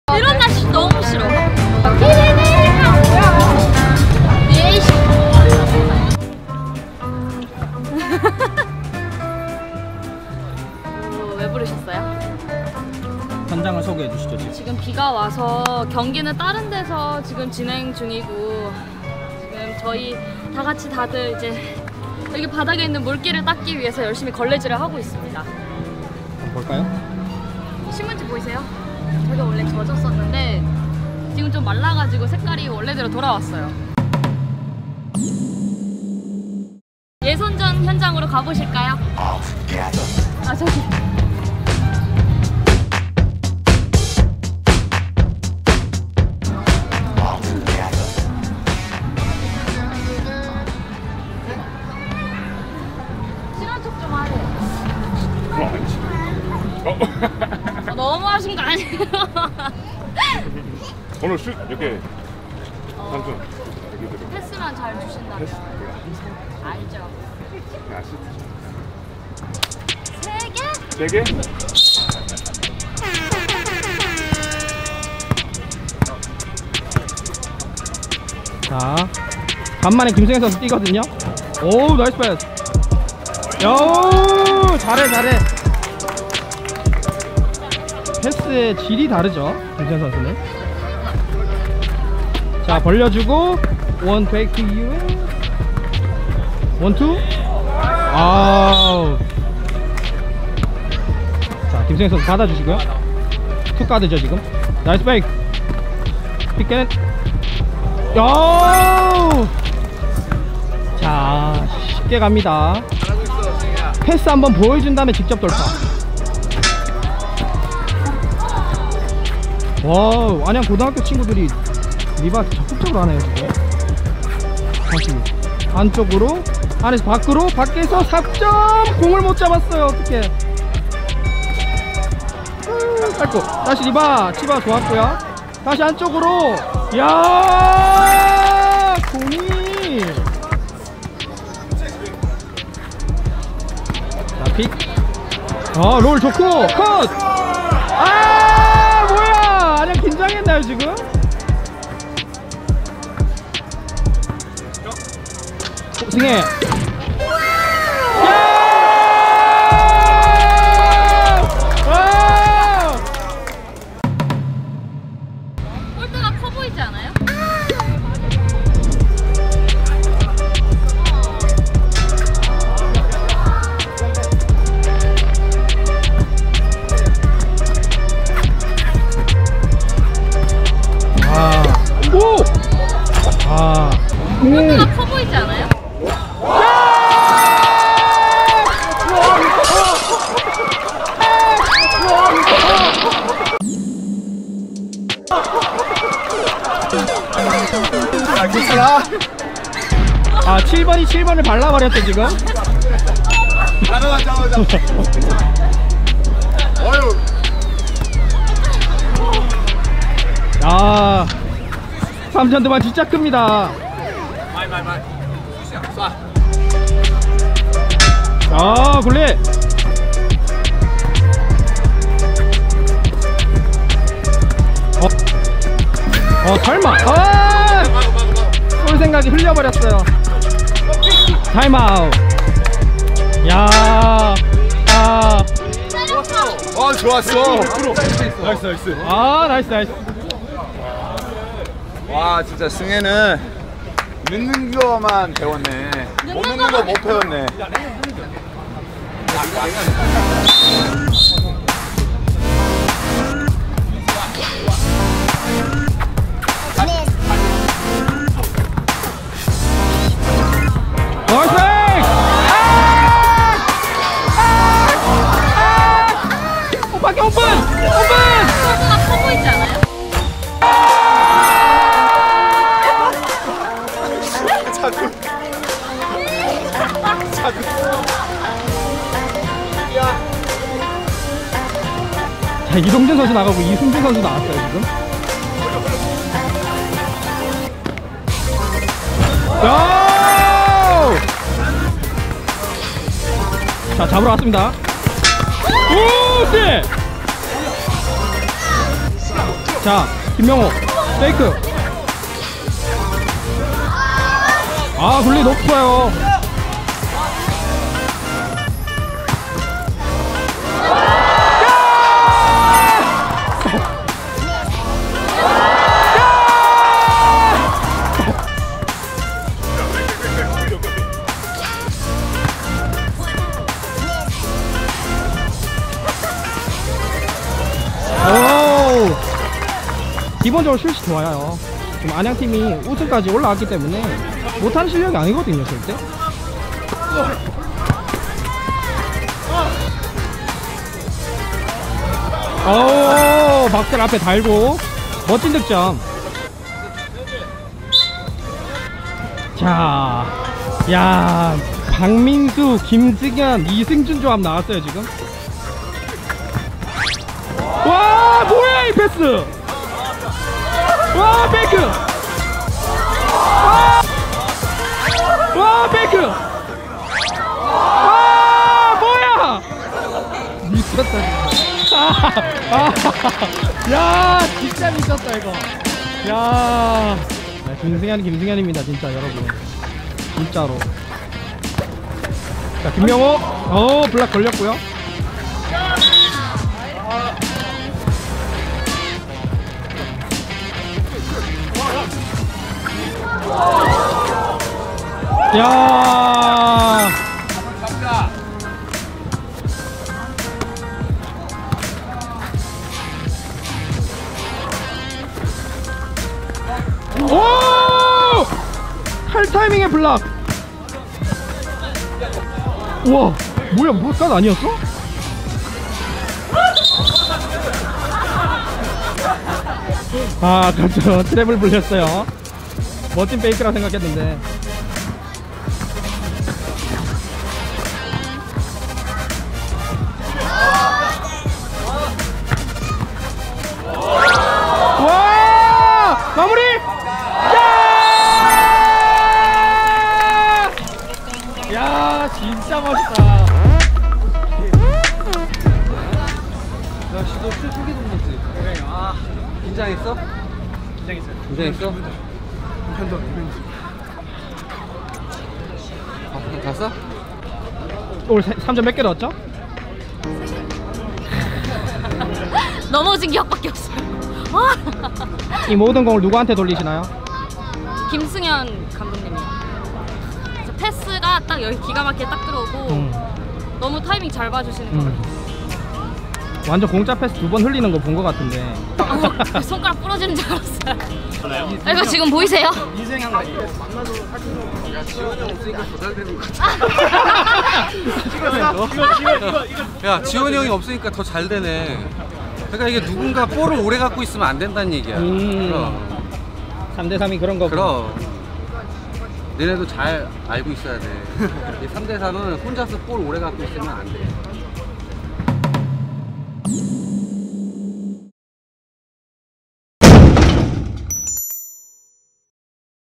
KBL 레전드 포인트가드가 3X3 뛰면 안되는이유. 패스가...
KBL 레전드 포인트가드 김승현 선수의 패스쇼!
잠자던 승혜도 깨우는 매직 핸드!!
사람잡는 세레머니까지 함께 확인하시죵~